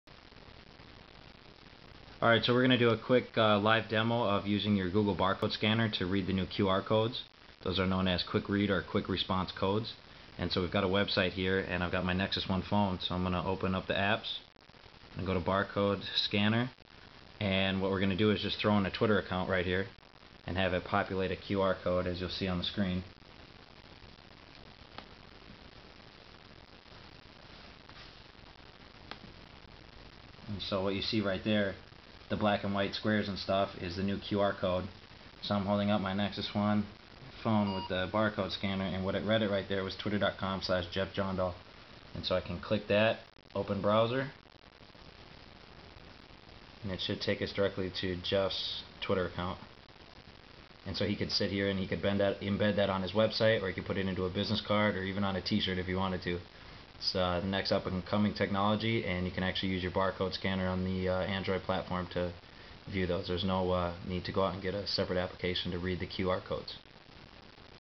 Live QR-Code Reading Demo Using Google Barcode Scanner on Nexus One .AVI
We stumbled upon a site that allows you to create your own QR (quick read or quick response) codes.  Curiously, I checked to see if the Barcode Reader on my Nexus One would also read the QR-codes...Yes, yes it did! I guess people with other cell phone platforms have to download a special app in order to read QR-Codes, but people lucky enough to be on Android do not have to put up with any awkward 3rd party apps to read the barcodes.

I had not seen any videos about this before, so my neighbor Jeph Jondahl and I shot a quick vid demoing the experience.